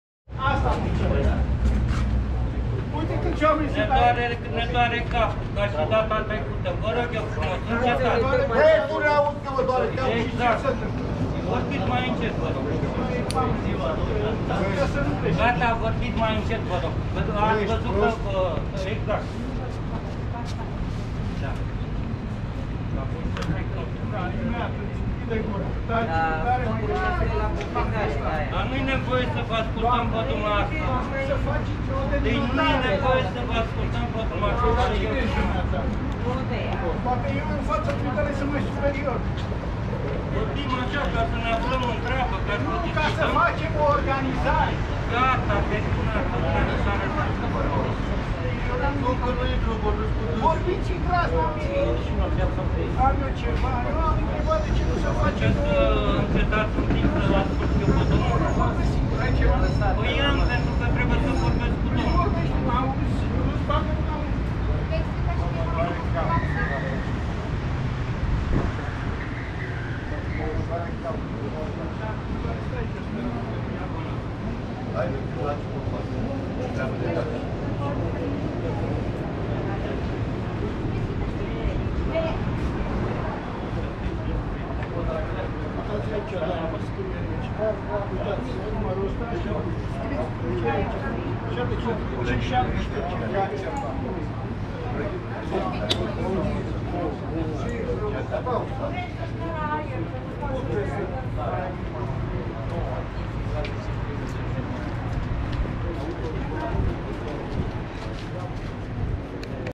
Ceartă la oficiul stării civile din sectorul 5, București - Curaj.TV
La cozile nelipsite și interminabile de la instituțiile de stat românii se împart în două categorii, cei care se revoltă și deranjații că cineva e revoltat/ă